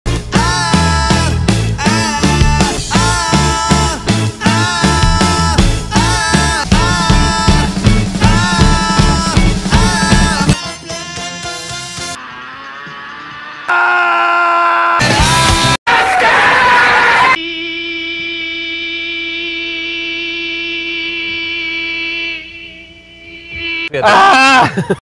TMBG but its just when they scream
The johns like to scream bloody murder alot